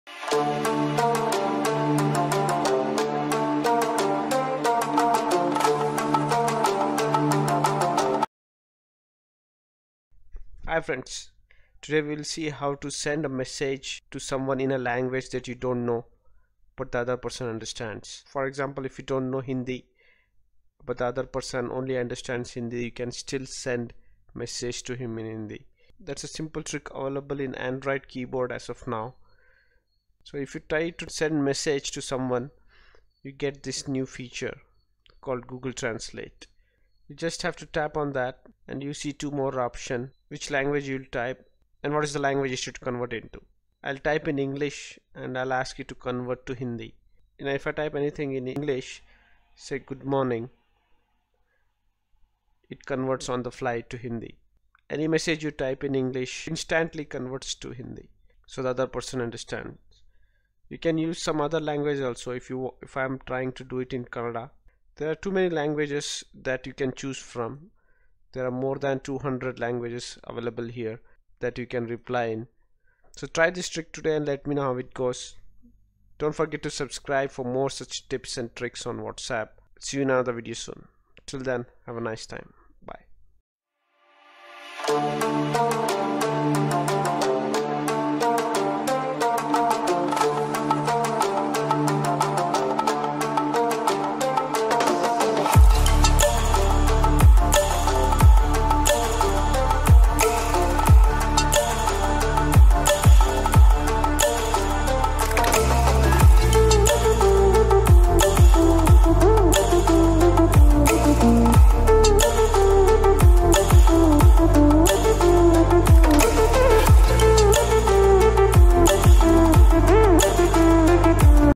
WhatsApp Trick || Send Texts in Different Languages Easily
Now you can send text easily in different languages without even knowing the language or having the keyboard installed to send. Just by using Google Translate built-in option.

The video shows how can you easily send text in other languages in WhatsApp without having to install other applications.

TrackID: AMLPK1154B20201020SRI